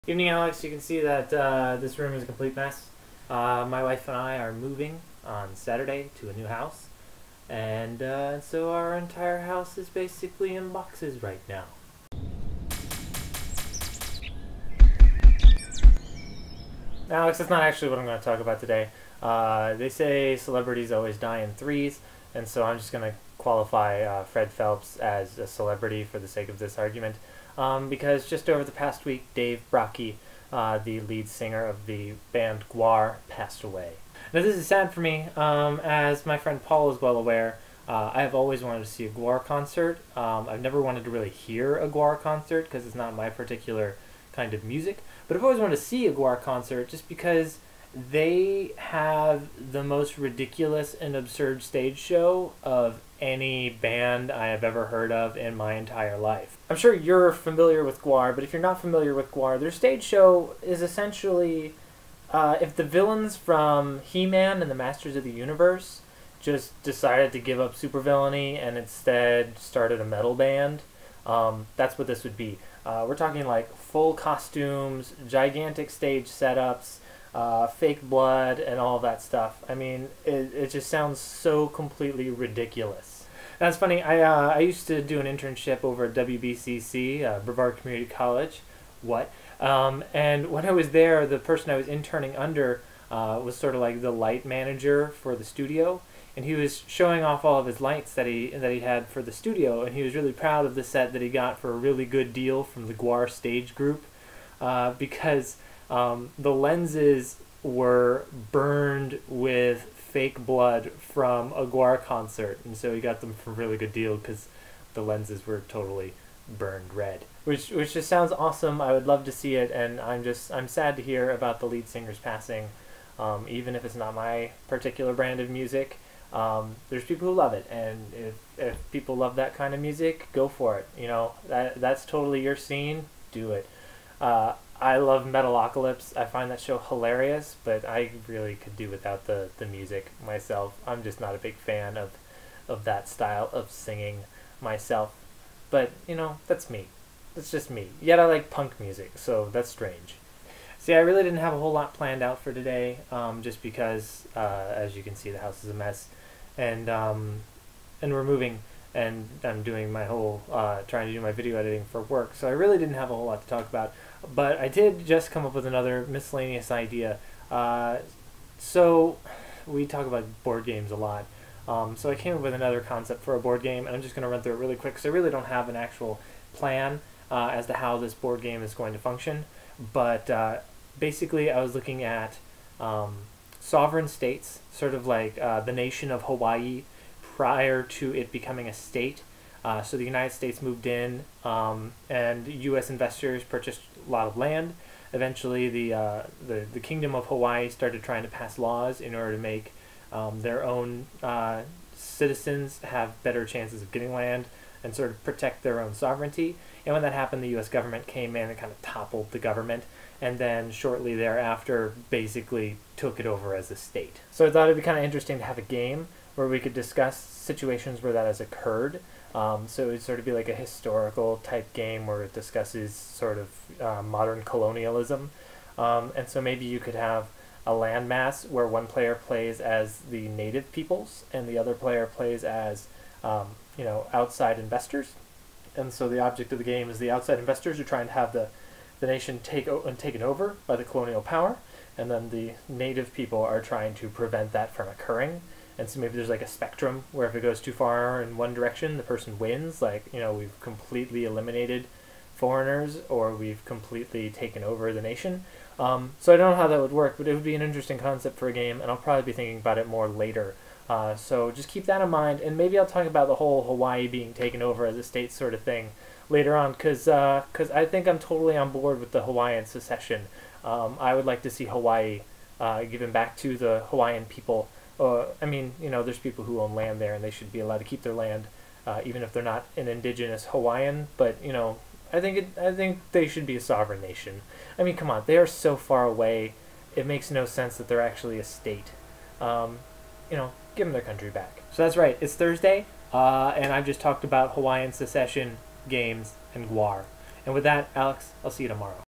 Dave Brockie and Hawaiian Sucession
Corey discusses the loss of Gwar's lead singer Dave Brockie and a general concept for a new game where players would fight for the future of a sovereign nation in Modern Colonialism, the game.

Have a project you want help with?  Leave us a comment or send us a Tweet!